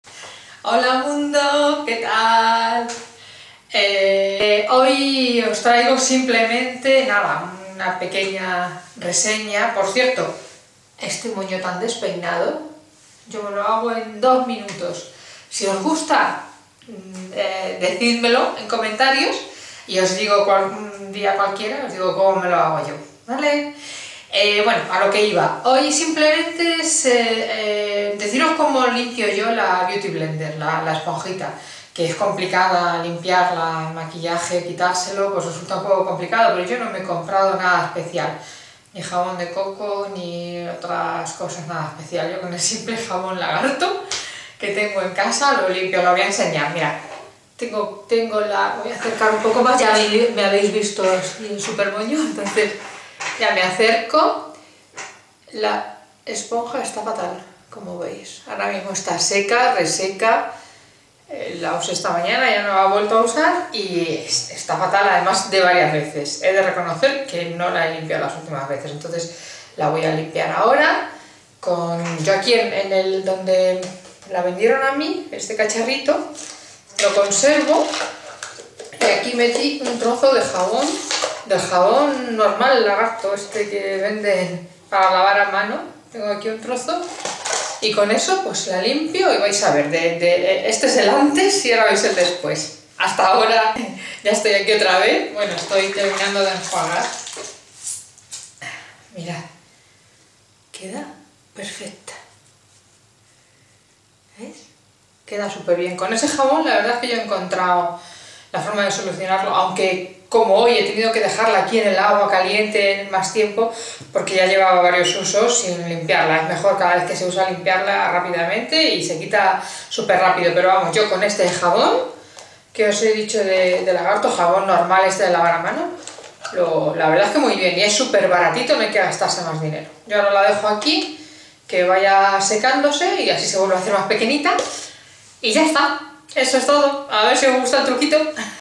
Truco para limpiar la Beauty Blender | Trick to clean the beauty blender | El mundo de Gracua
Con el jabón de castilla es muy fácil mantener limpia la esponja de maquillaje beauty blender. Os cuento como lo hago yo. Espero que os sirva¡!